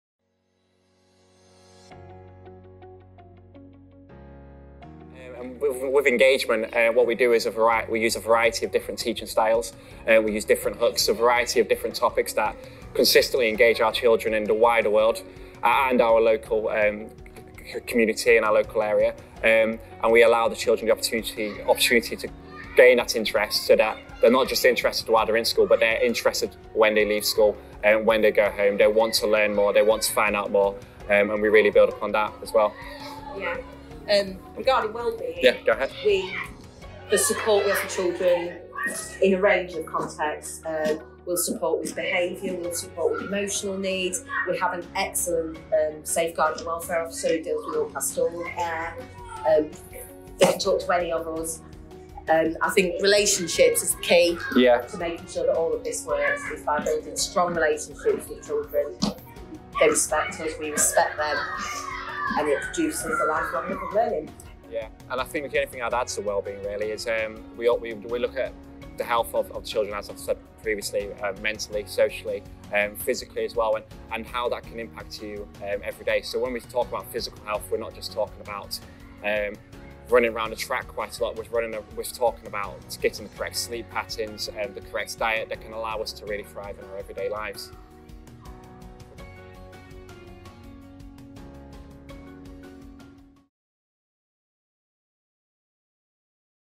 Engagement and Wellbeing at White's Wood Academy
Keeping children engaged and looking after their wellbeing are vitally important to the teachers at White's Wood Academy.